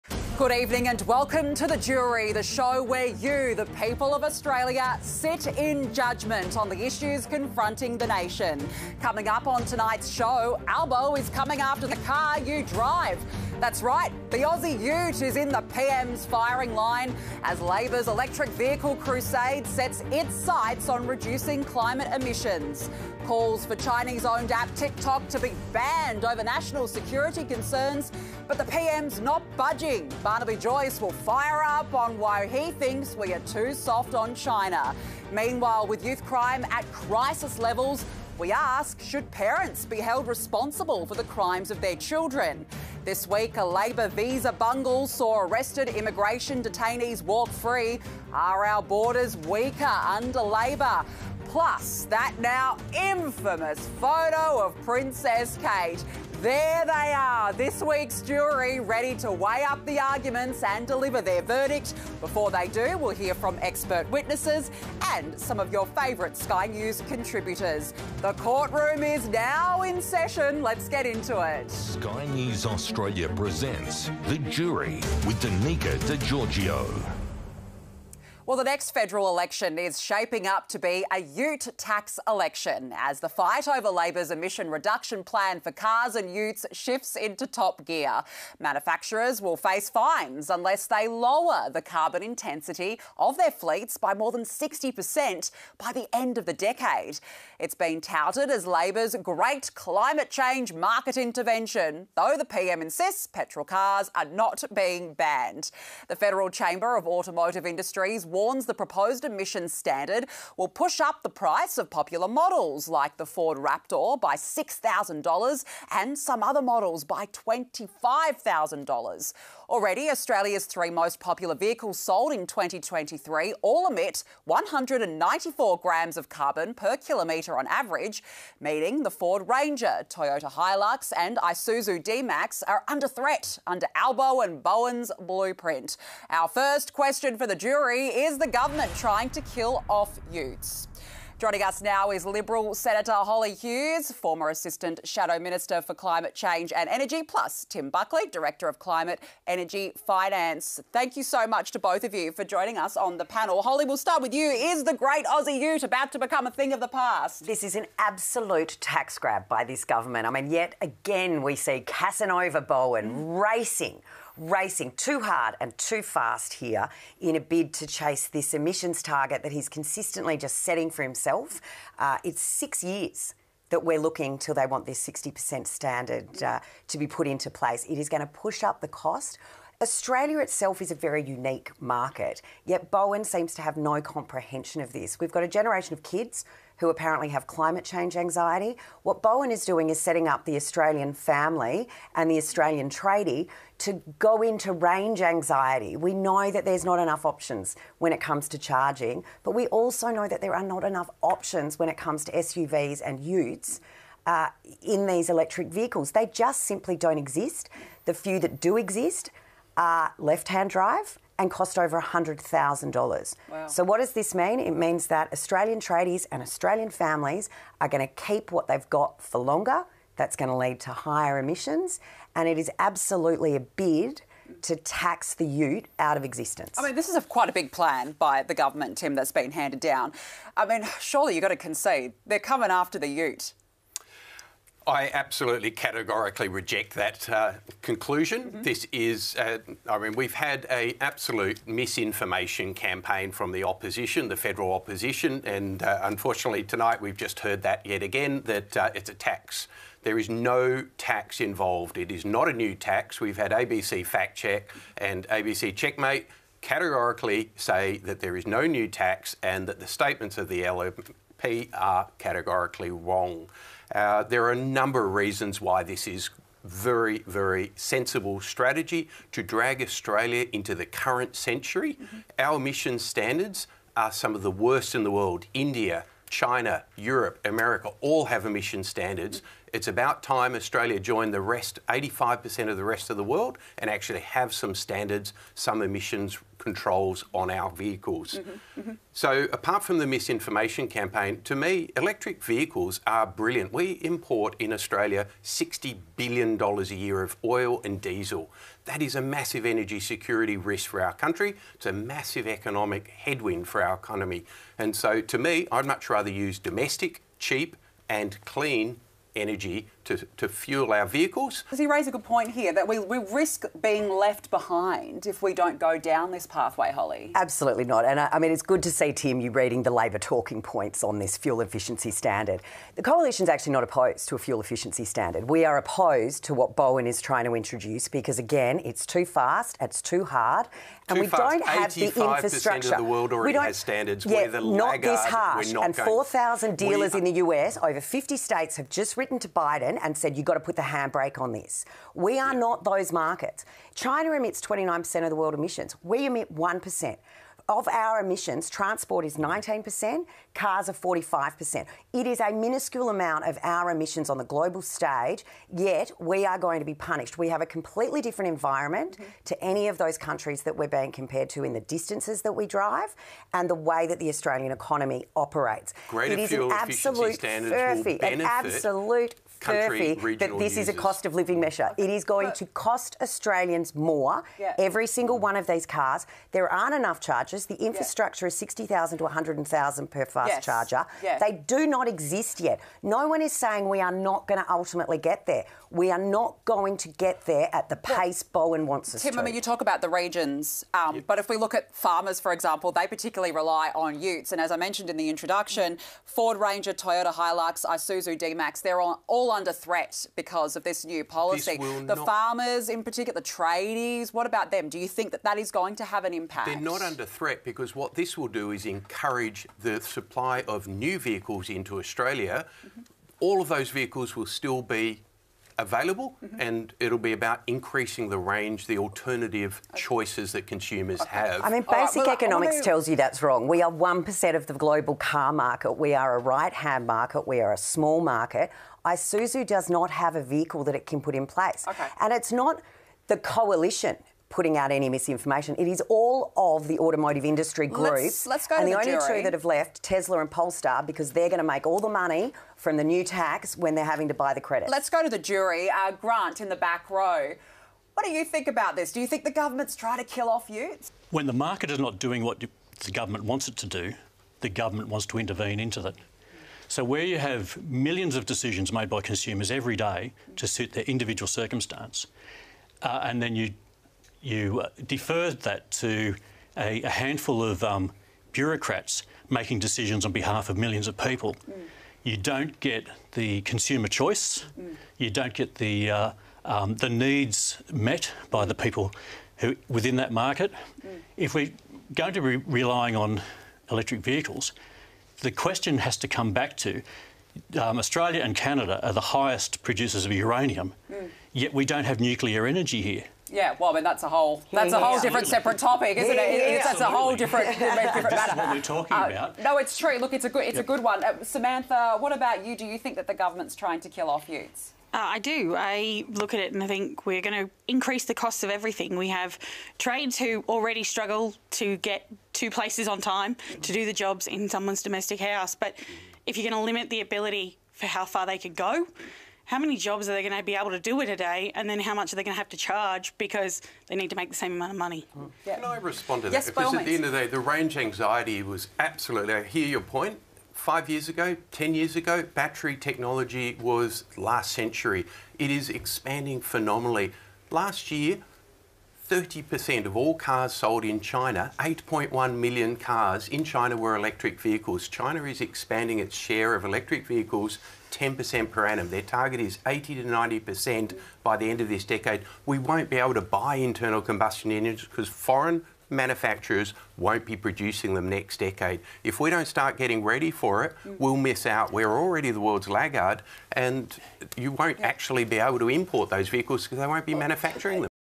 Albanese ‘insists’ cars are not being ‘banned’ in emission standards market intervention
Sky News host Danica De Giorgio says Labor’s emission standards are set to be fought over in the next federal election as Prime Minister Anthony Albanese “insists” petrol cars are not being “banned”.

“The fight over Labor’s emission reduction plan for cars and Utes shifts into top gear,” she said.

“Manufacturers will face fines unless they lower the carbon intensity of their fleets by more than 60 per cent by the end of the decade.

“It has been touted as Labor’s great climate change market intervention.

“Though the PM insists petrol cars are not being banned.”